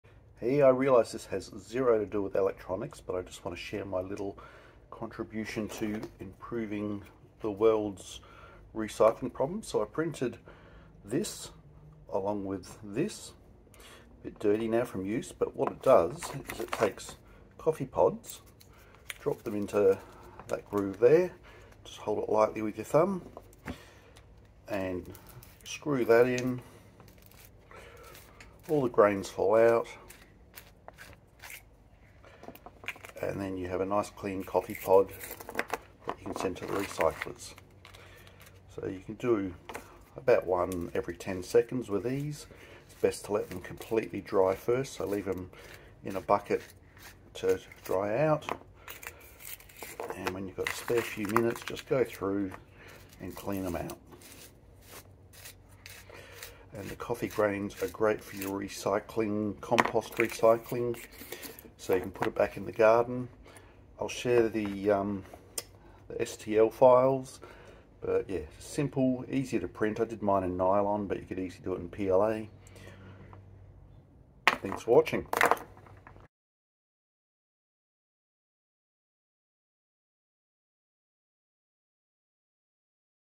3D Printed Coffee Pod Recycling Tool
This 2 part tool will clean out your Nespresso compatible pods ready for recycling.
Did you know that the pods go straight to the landfill if you have not emptied them?